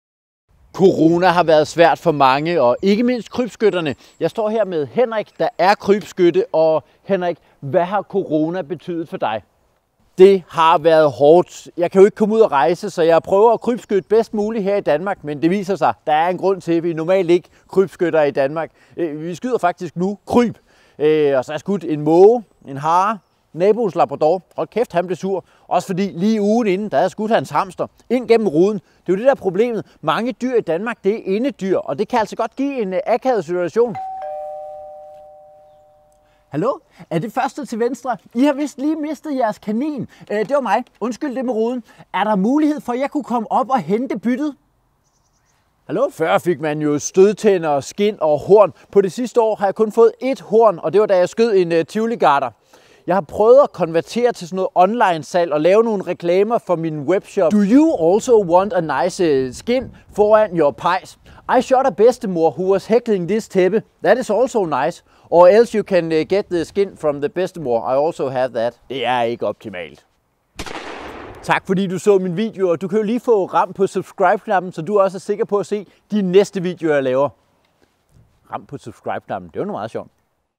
Hjemsendt krybskytte 🦁
Corona har været svært for alle. 🤷‍♂️
Og ikke mindst krybskytter. 🐘

Hvordan får man det til at løbe rundt når man ikke kan rejse ud og skyde truede dyr? 🤔
Støt Henrik ved at se reklamen for webshoppen til sidst.